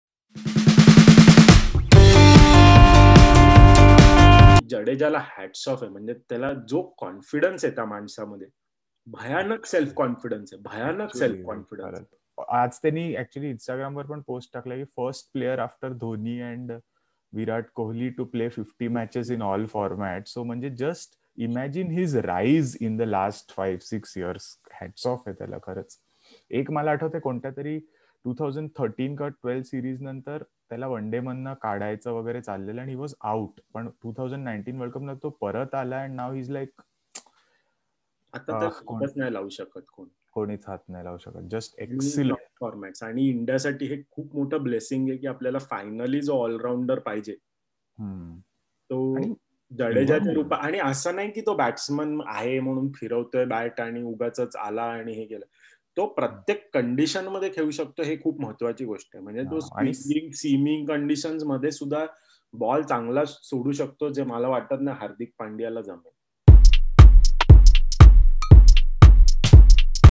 Ravindra Jadeja | The best all-rounder across formats | Marathi Cricket | Jadeja rocks at the MCG.
We rave about Jadeja and his magnificent performance in the BOXINGDAY test.